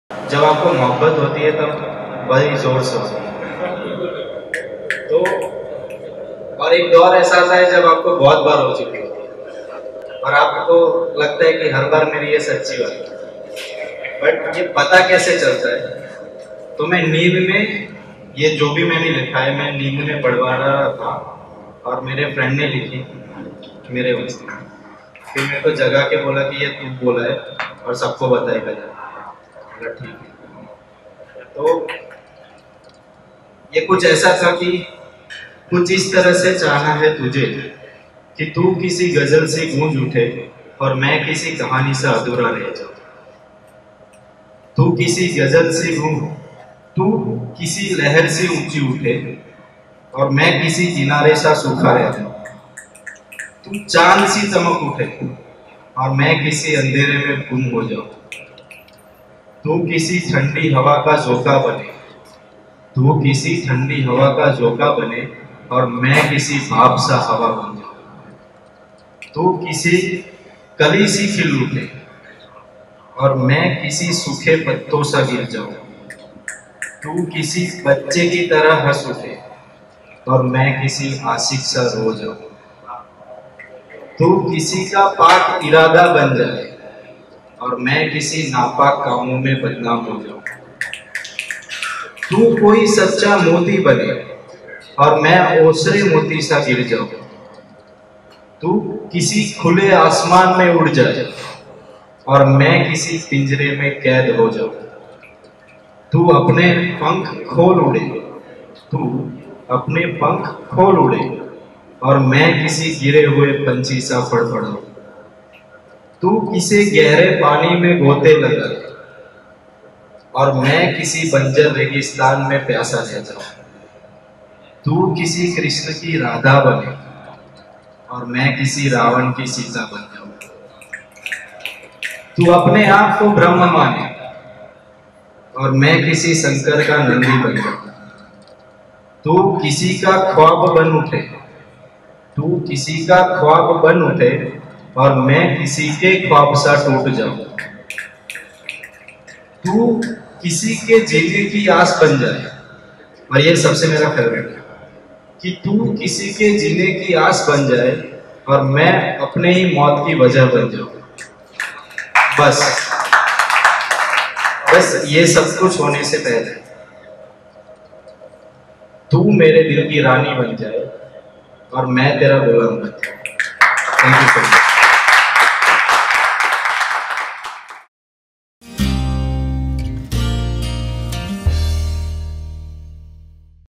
Short Hindi Poetry Video by Bhavik at Nojoto Open Mic Ahmedabad | Heart Touching Poetry
Short Hindi Poetry Video by Bhavik at Nojoto Open Mic Ahmedabad | Heart Touching Poetry.Do watch this amazing Short Hindi poetry on Love and don't forget to share this heart touching sad Hindi Love poetry with your friends.